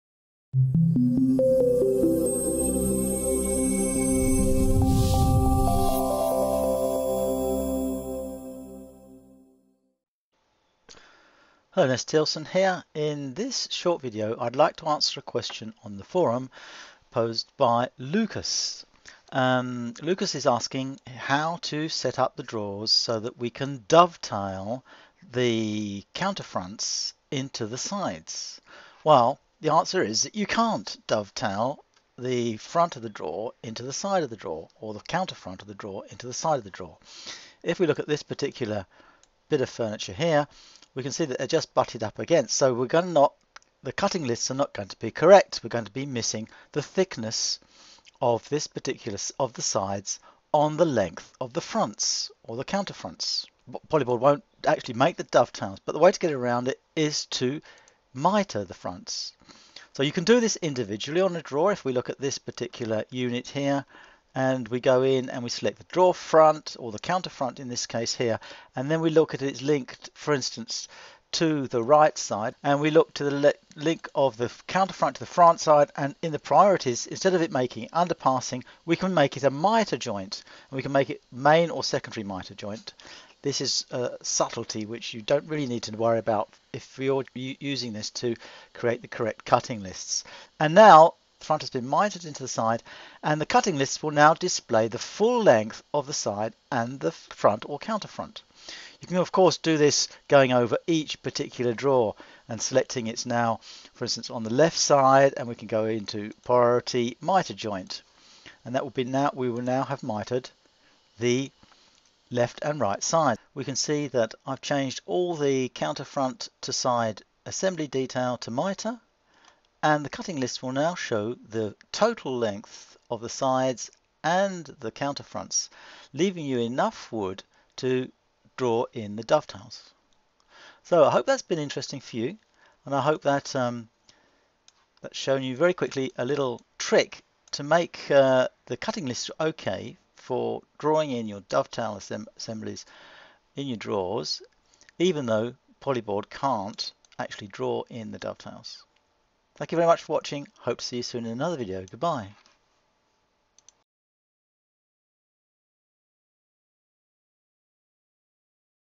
Dovetail Drawers in Polyboard
You cannot strictly speaking set up dovetail drawers in Polyboard, but you can use mitre joints to obtain the correct cutting lists.

Click here to view a second video that shows you how to use Polyboard's Manufacturing Methods to apply this type of construction detail automatically